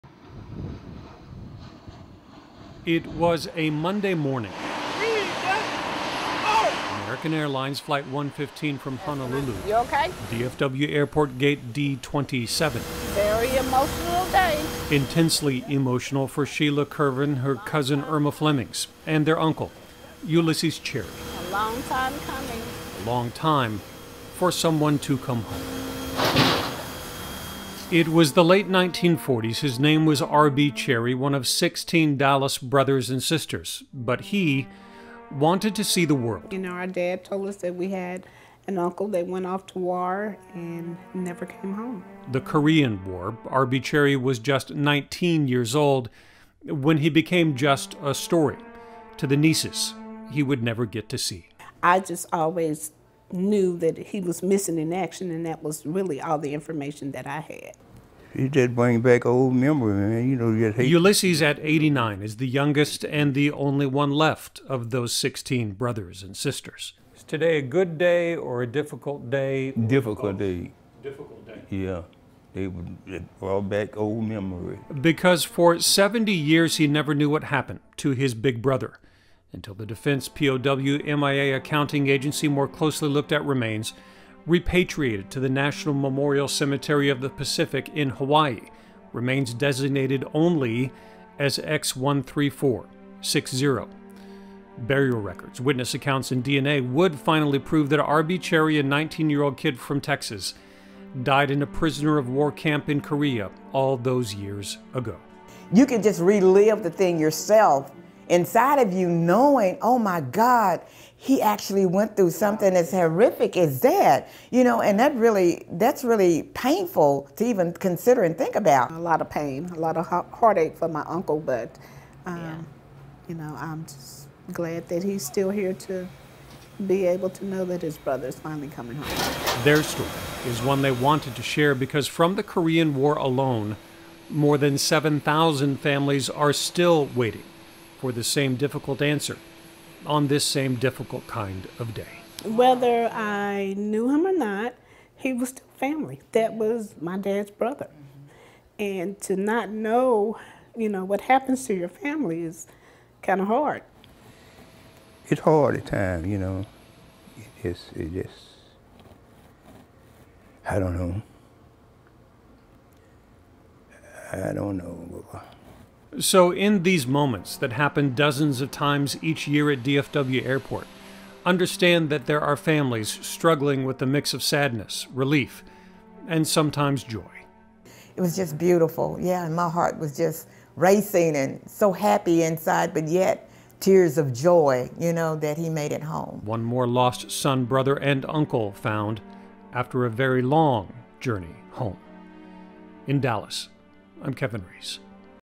A Korean War POW and the 72-year journey home to Dallas
R.B. Cherry was only 19 when he disappeared during the Korean War. Now, with the mystery finally solved, his family finally gets a chance to say goodbye on Friday.